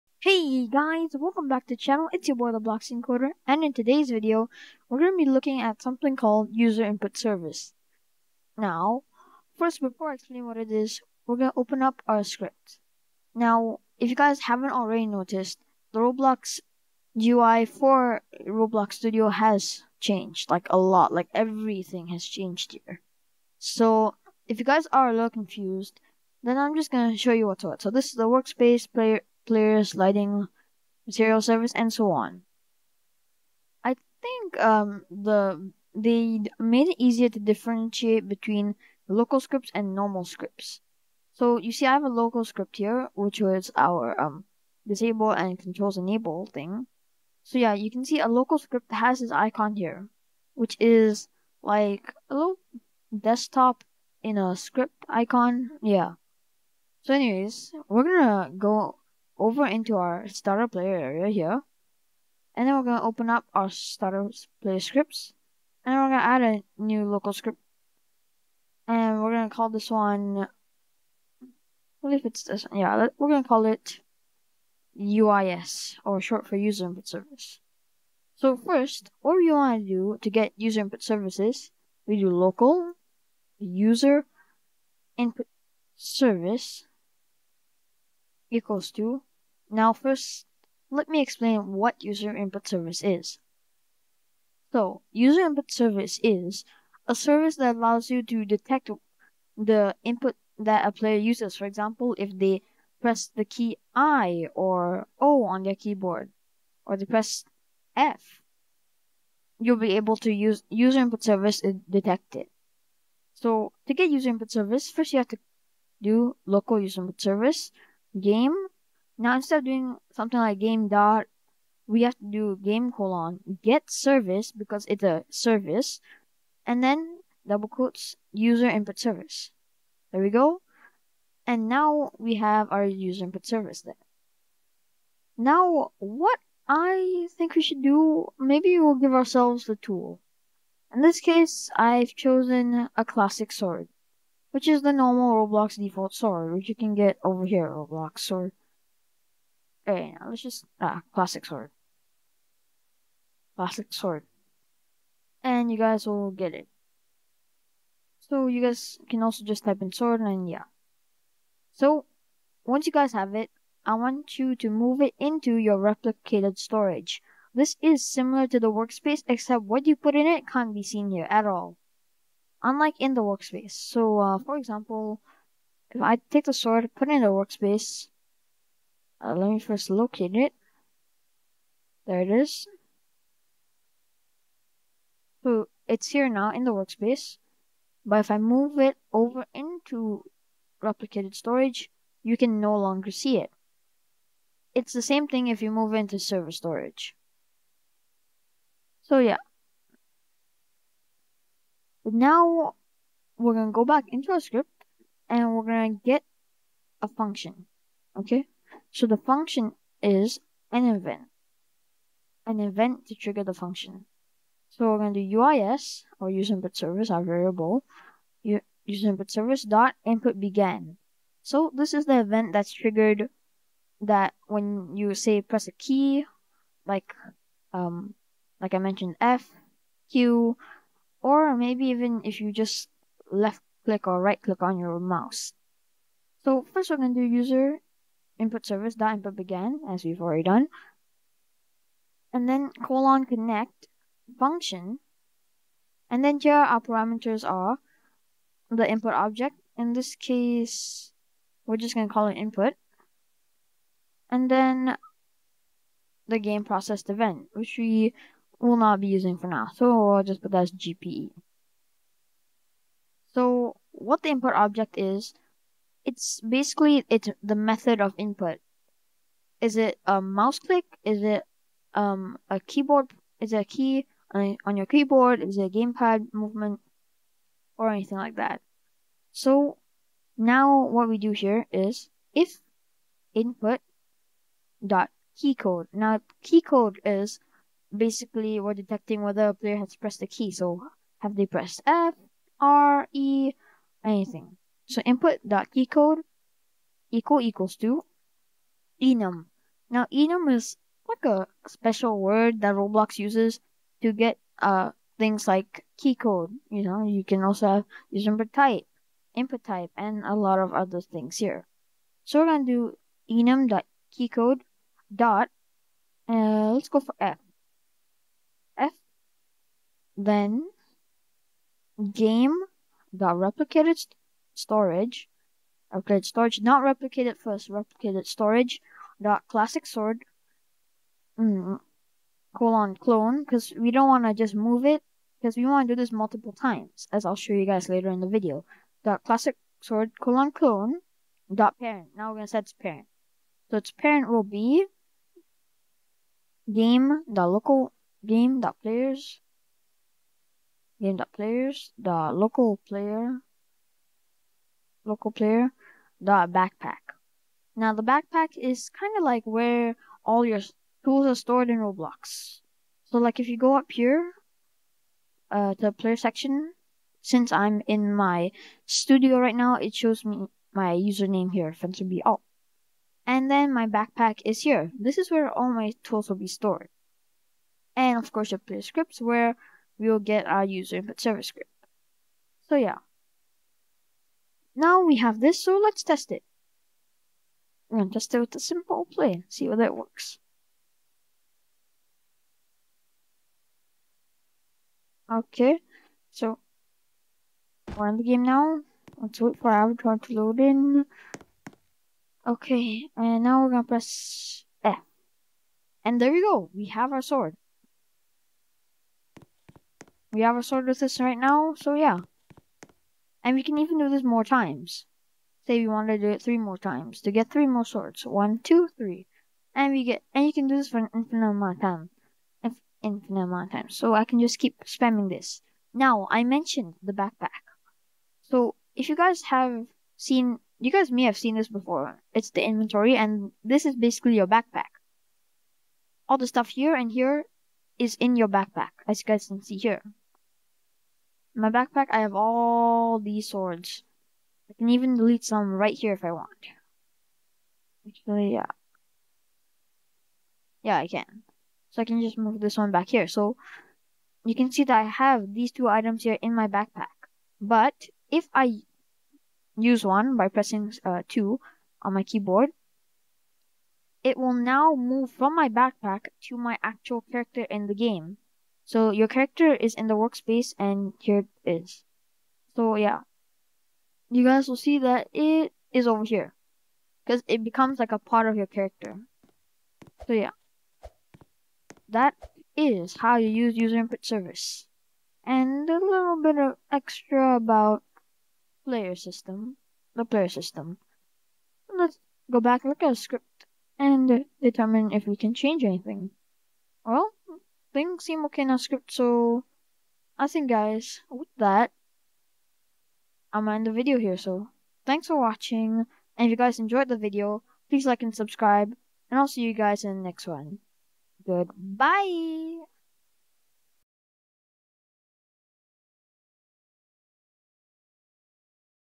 How to DETECT A PRESSED KEY on ROBLOX STUDIO (UserInputService)
In today's video, we will be looking at the UserInputService! This allows you to detect when a player presses a key on their keyboard!